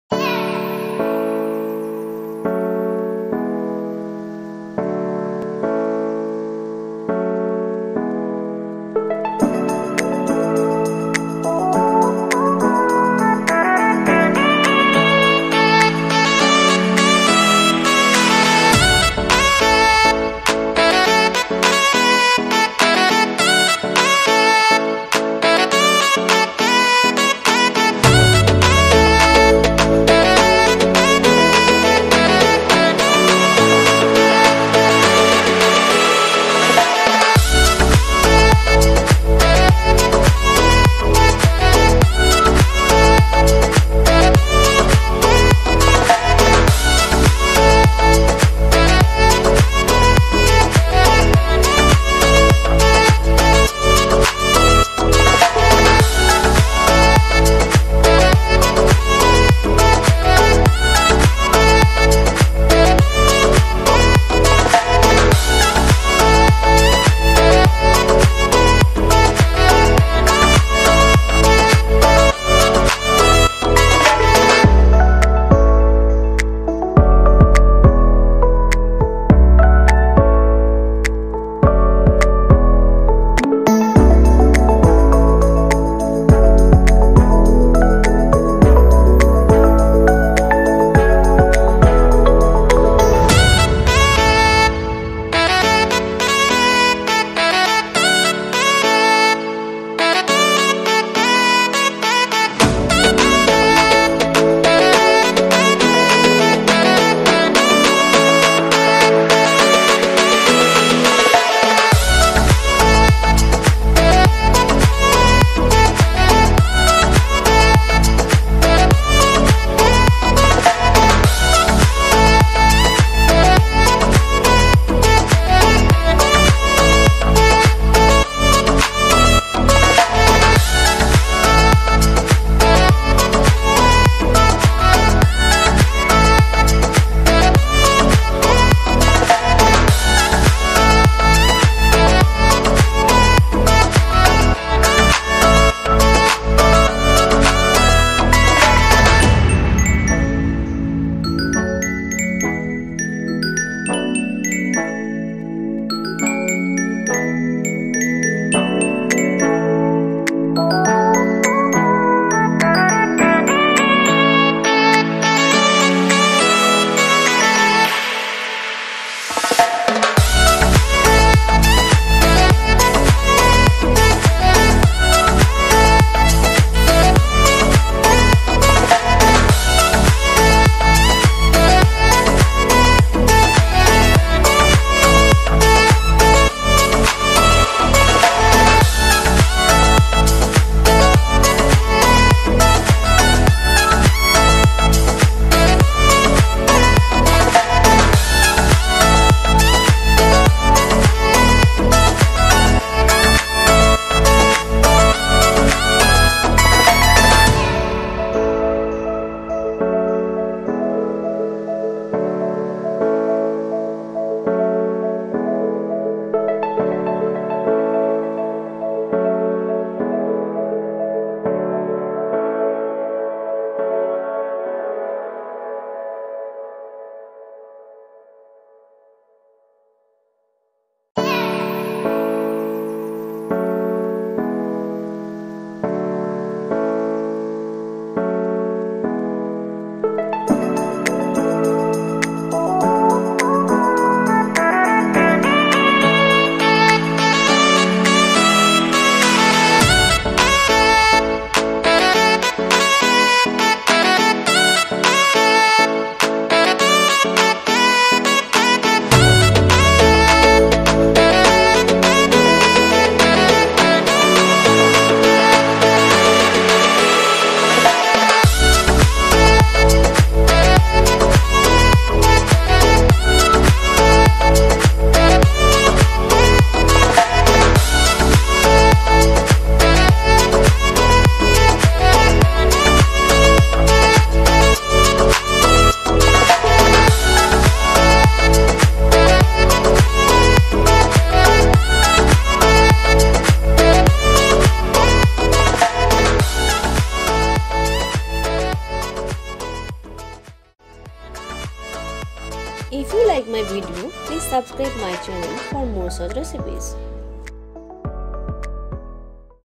#91 Make Perfect Vegetable Pulao in Kadhai under 15 Minutes! ~ From Kitchen Special
Musician: @iksonmusic